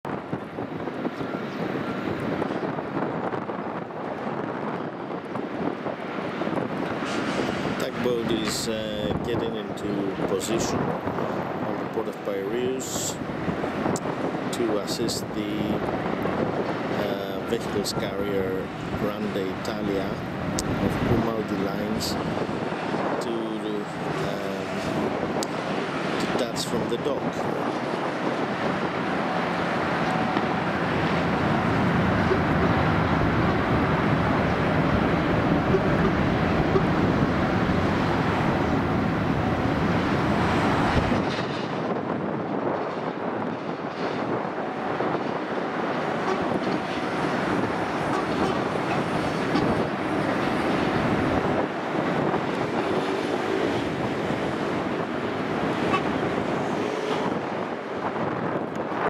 Grande Italia Vehicle Carrier of Grimaldi Lines is departing from the Port of Piraeus, Greece
with the assistance of Tug boat Christos XL II